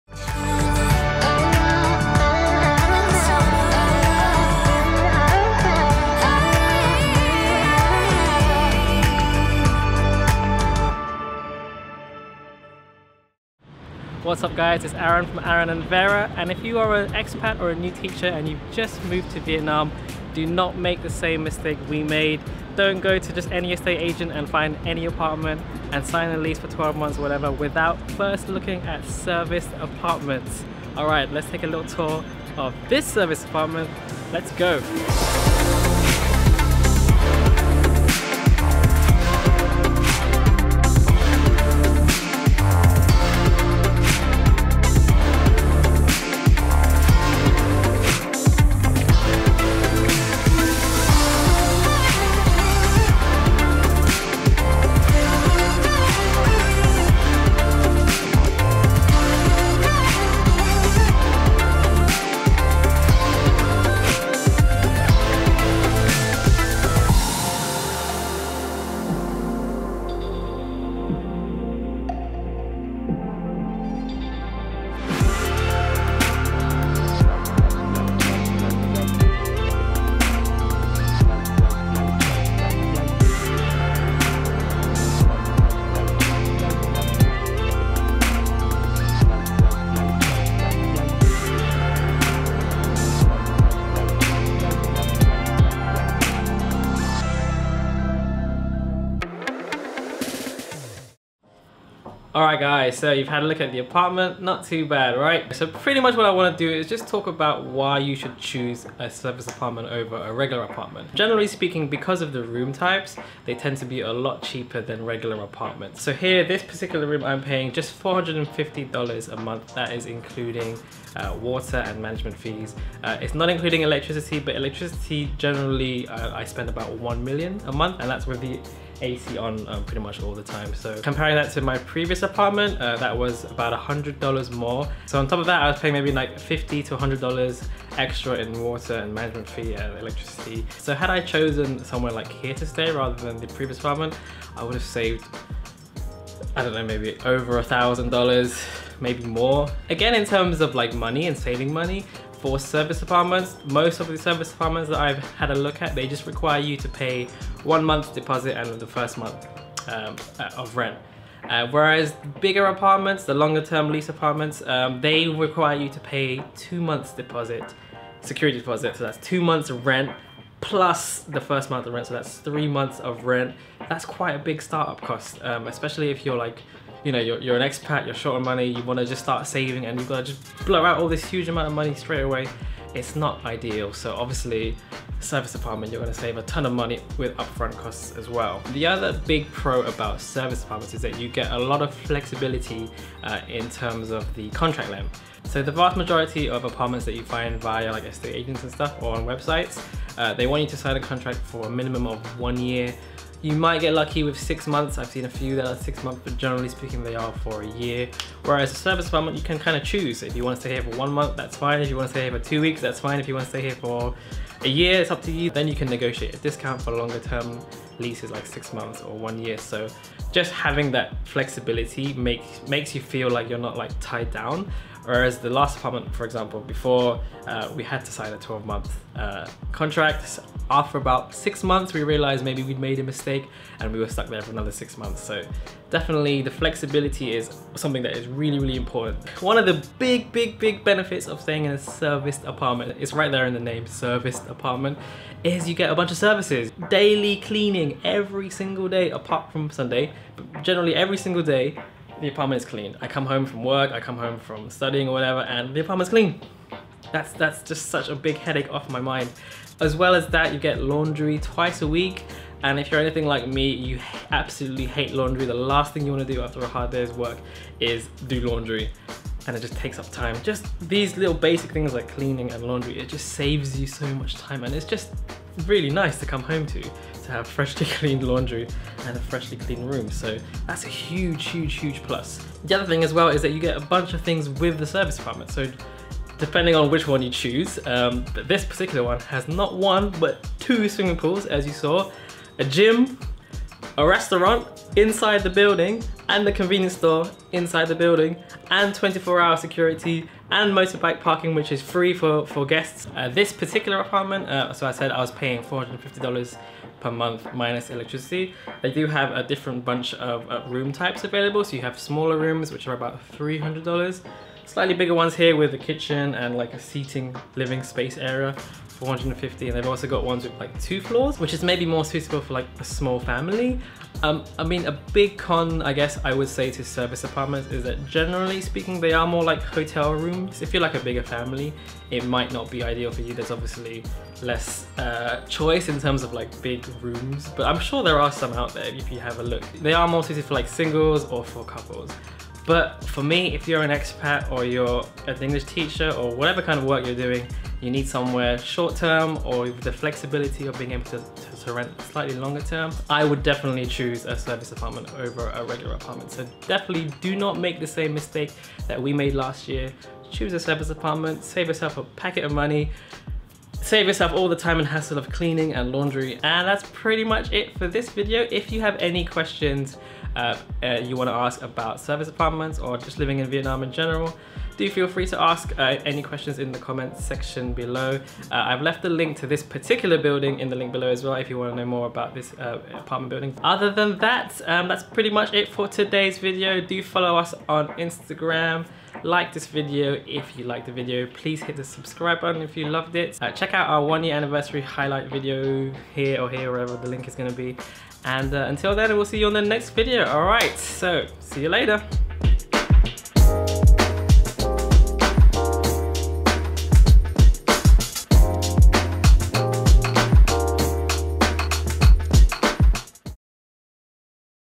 Serviced Apartment Tour for $450 | Ho Chi Minh City (Saigon), Vietnam
UPDATE - I stayed here twice again in 2019 and early 2020 and unfortunately the standards have definitely gone down. The staff are nice and very helpful, but the electricity price was way above average, and there were constant building works and very noisy parties/karaoke every night. I also felt that the type of people staying there were mostly idiot stereotypical gap-year/party travel types, dodgy-looking women and overall it didn't feel very "family-friendly" like it did when I made this video. If you are looking for a short-term stay in D7, it's still an okay choice, but I'd look elsewhere if you need to stay in D7 long-term. You can definitely get more for your money elsewhere in the city if you're not set on D7.